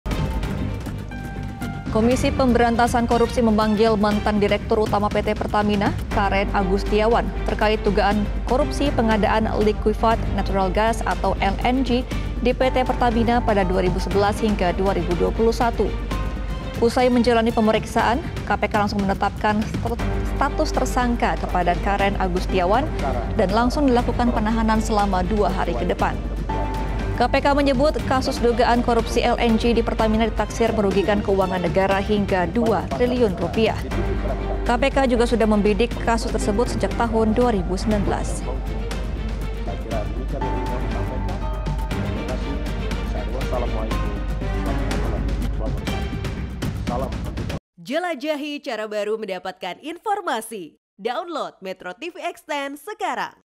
Eks Dirut Pertamina Karen Agustiawan Jadi Tersangka Kasus Korupsi Pengadaan LNG
MetroTV, Eks Dirut Pertamina Karen Agustiawan Jadi Tersangka Kasus Korupsi Pengadaan LNG